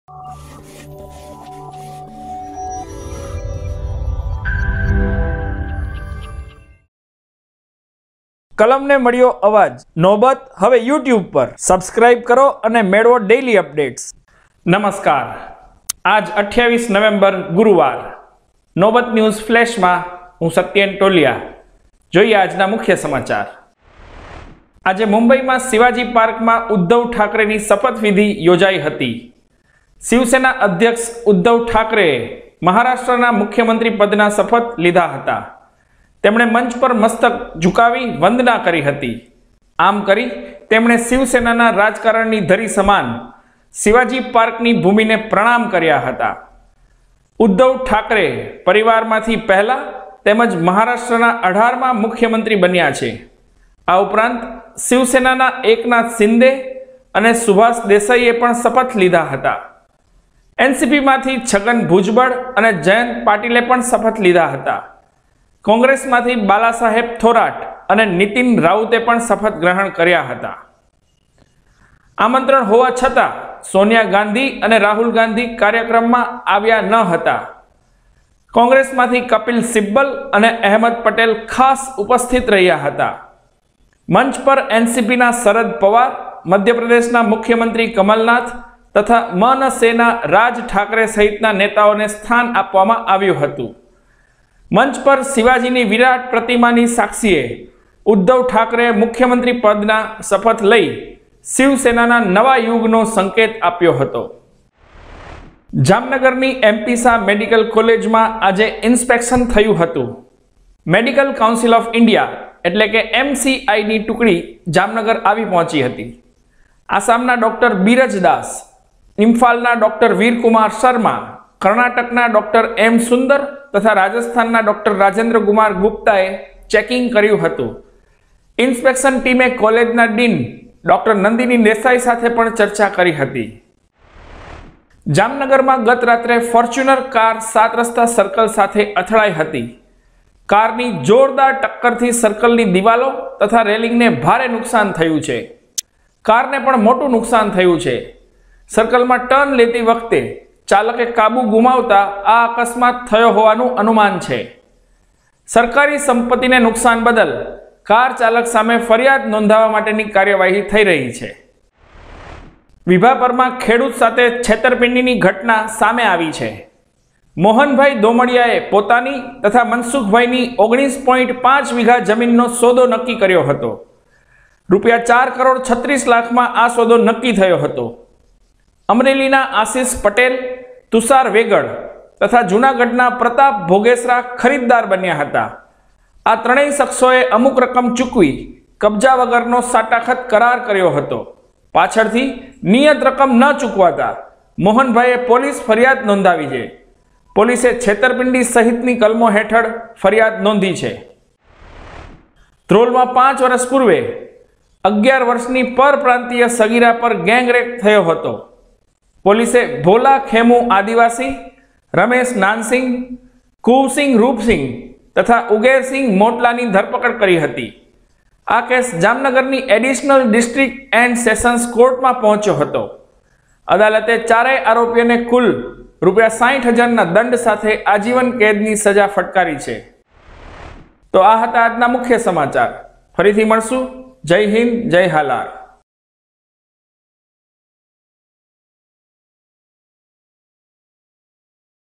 News Flash: Uddhav Thackeray takes oath as Maharashtra's CM
1. Uddhav Thackeray takes oath as Maharashtra's CM
2. MCI's Inspection in Jamnagar's M P Shah Medical college
3. Car crashed in to saat rasta circle in Jamnagar
4. Farmer got cheated in a Land deal in Vibhapar
5. Four accused gets lifetime imprisionment in a Gang Rape case in Dhrol